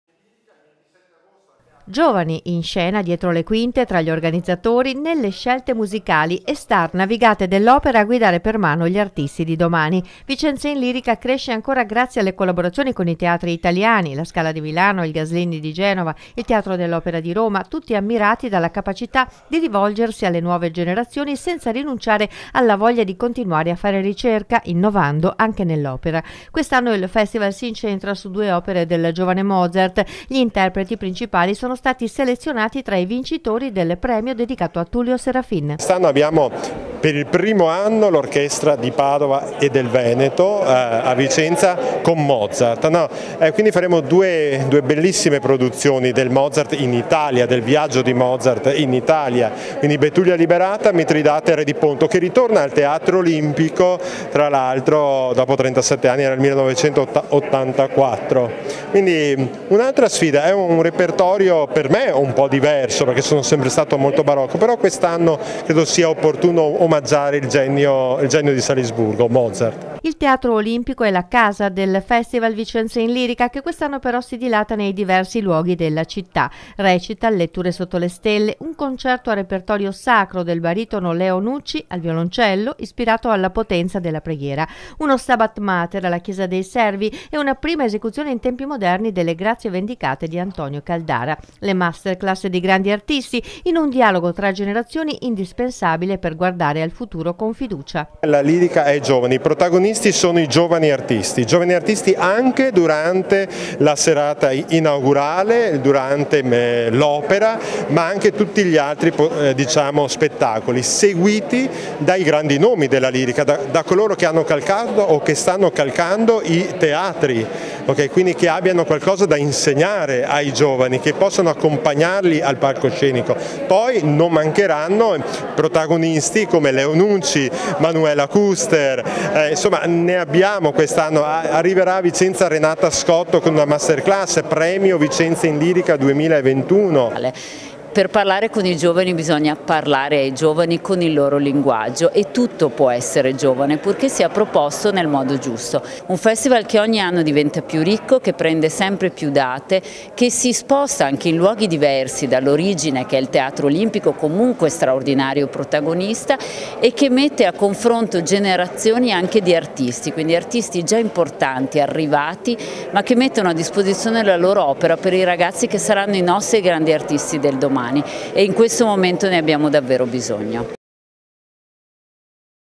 LIRICA PER GIOVANI
Torna Vicenza in Lirica al Teatro Olimpico con i vincitori del Concorso Tullio Serafin, che saranno gli interpreti delle due nuove opere in cartellone. La lirica è giovane, lo slogan di quest'anno.